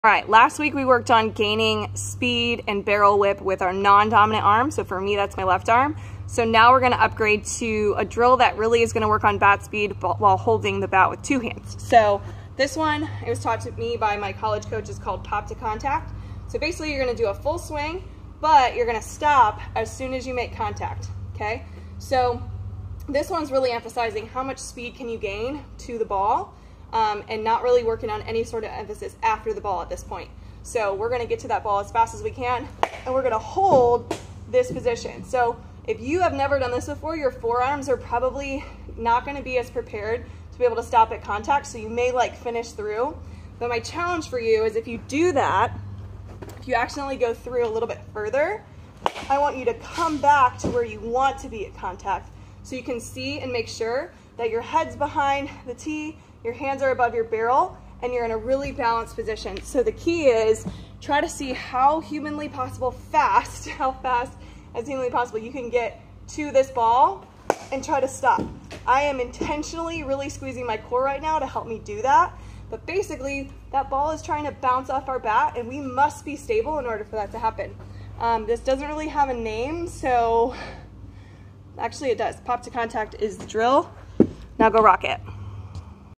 Barrel whip: pop-to-contact
Last week, we worked on some one arm strengthening, now let's get those forearms strong so that ball can JUMP off our bat at contact! 💥

🔖 find all things swing development under my "Hitting Drills" series!